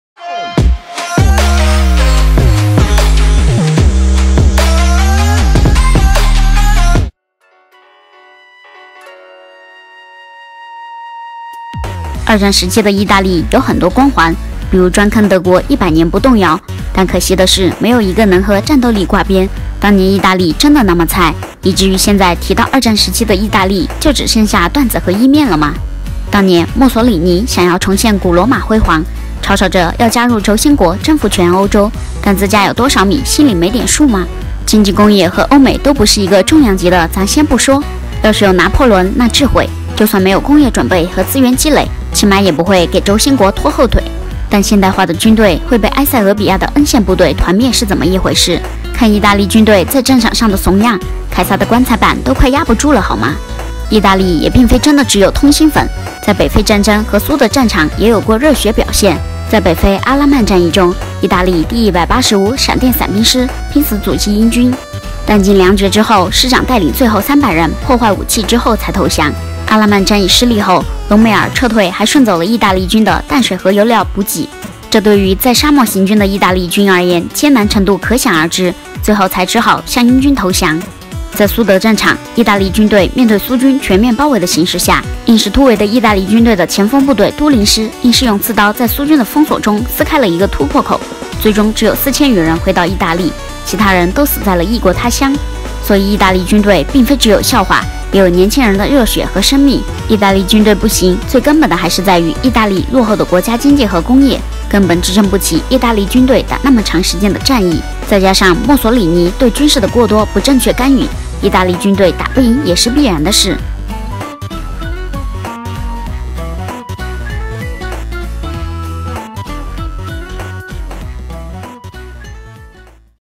二戰時，戰力最被低估的一個國家，不是中國而是它
二戰時期的意大利有很多光環，比如“專坑德國一百年不動搖”“但可惜的是沒有一個能和戰鬥力掛邊。當年的意大利真的那麽菜，以至於現在提到二戰時期的意大利就只剩下段子和意面了嗎？